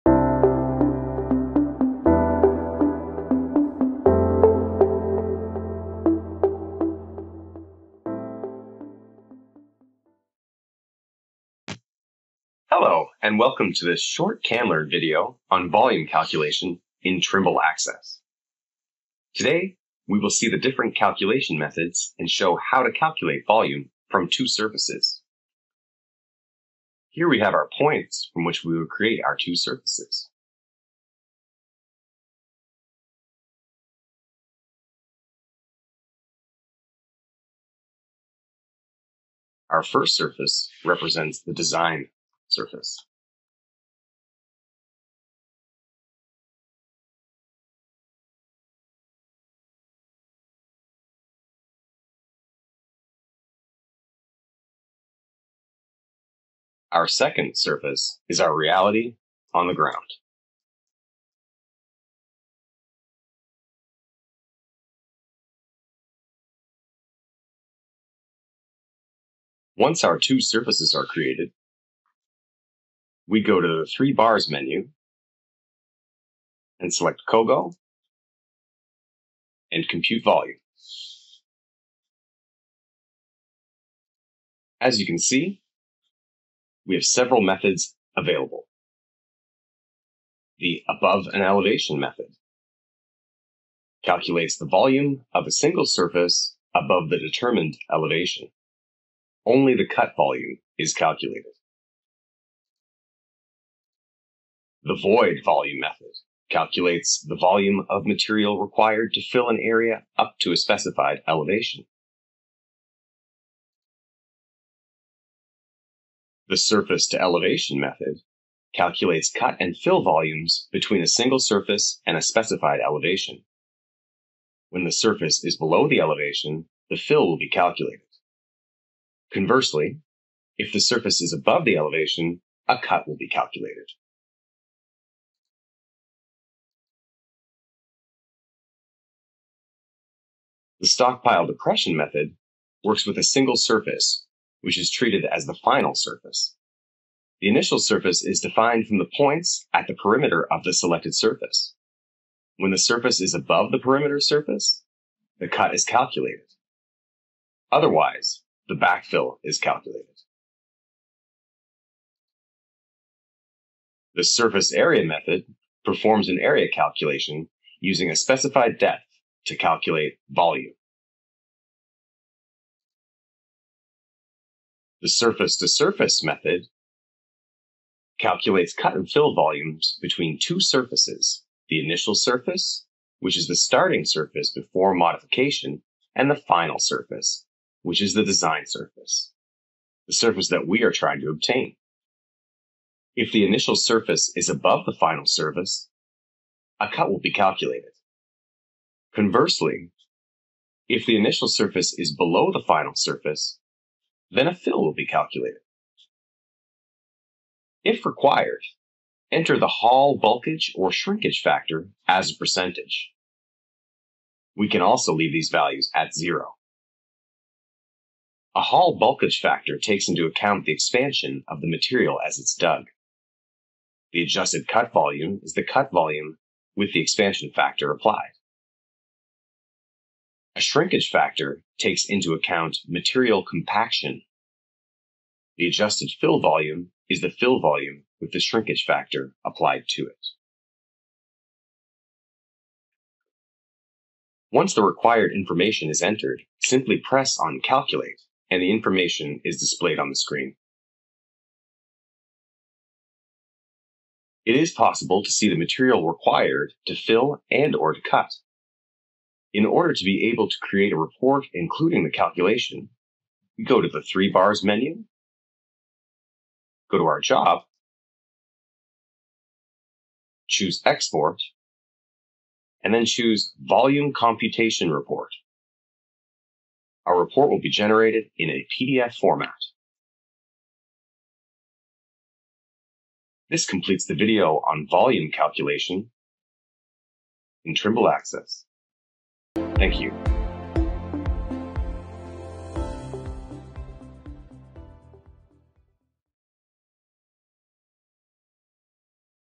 Can-Learn: Volume Calculation in Trimble Access
In this video, Matt Burns will explain the different volume calculation methods in Trimble Access and you how to calculate volume from two surfaces.